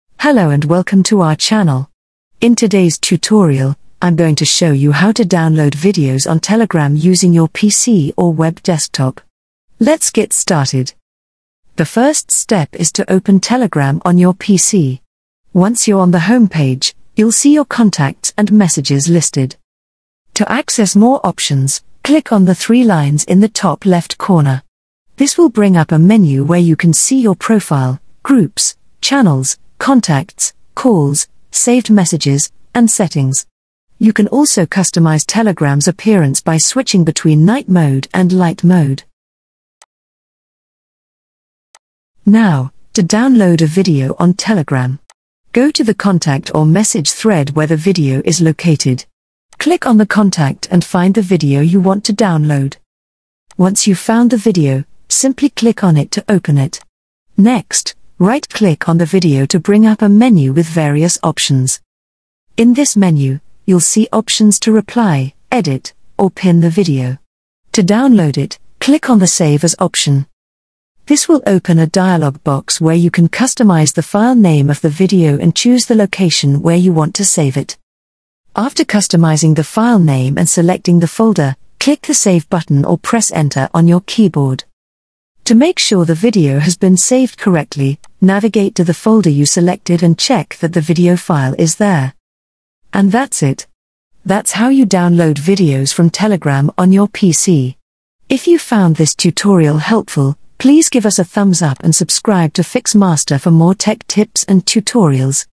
How to Download Videos on Telegram PC/Laptop/WEB | Save Video
Learn how to download videos on Telegram directly to your PC, laptop, or web browser without using third-party apps or websites! In this tutorial, we'll guide you through the process of saving videos from Telegram using the official Telegram app. Discover how to easily access and download videos from Telegram on your desktop or laptop for offline viewing. Whether you're a Telegram user looking to save important videos or simply want to enjoy content offline, our step-by-step guide has you covered. Watch now and start downloading videos on Telegram hassle-free!